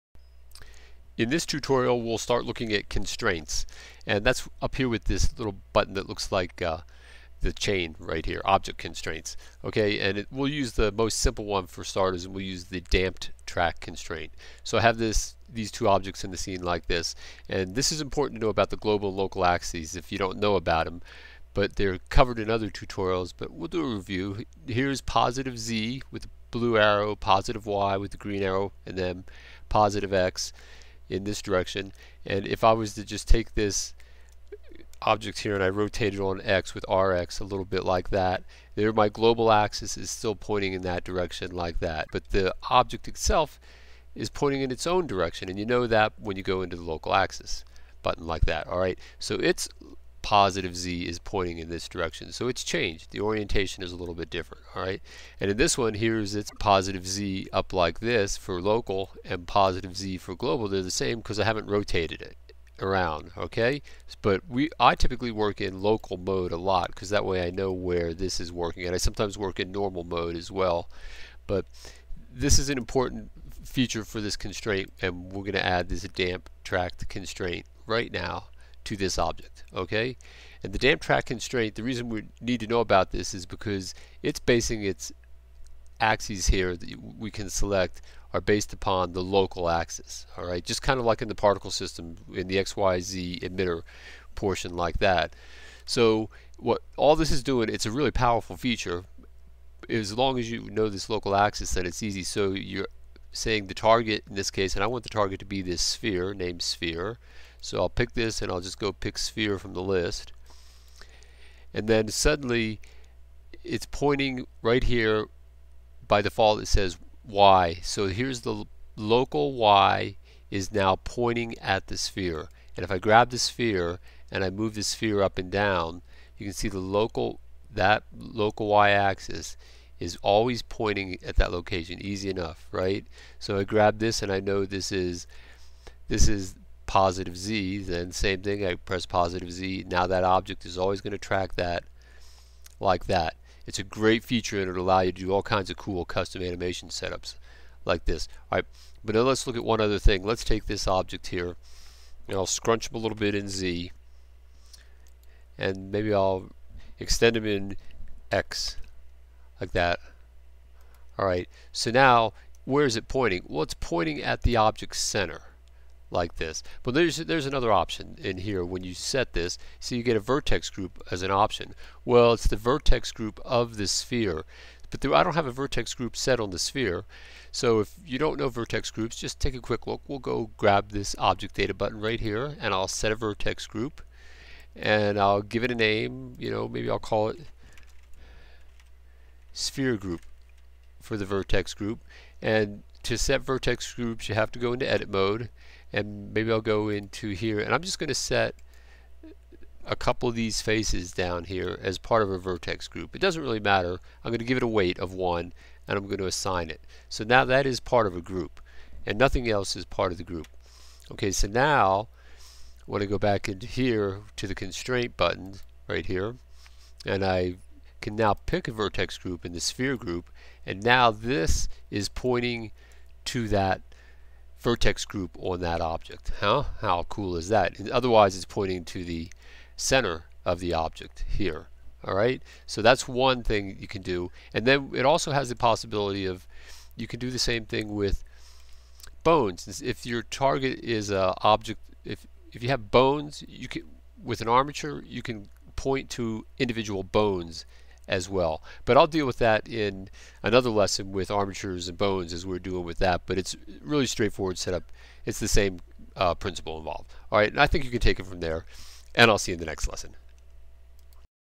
Blender Tutorial - Damped Track Constraints and Vertex Groups
In this tutorial for new Blender users, we look at the very useful damped track constraint.  It's a great tool for having one object track another. Blender 2.65.